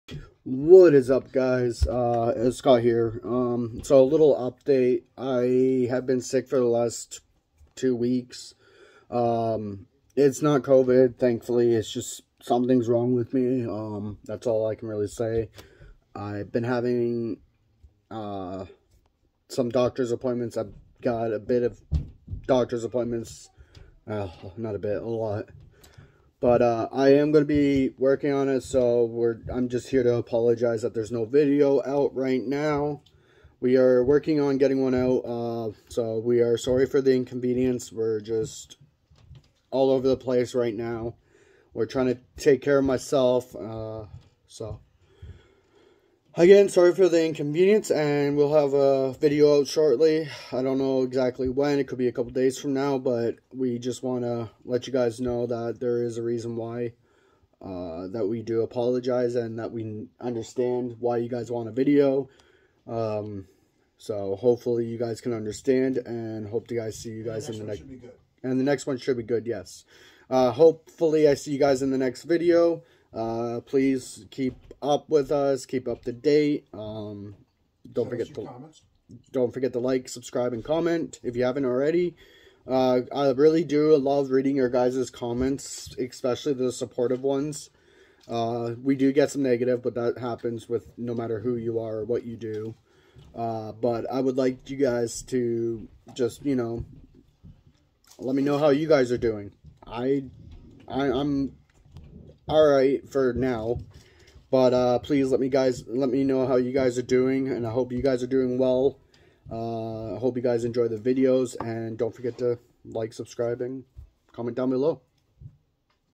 little update on why we haven't uploaded yet.
Hey guys/girls,

So I have been sick for the last two weeks. I have been extremely busy with doctors appointments. I do hope you all understand and wish you all the best.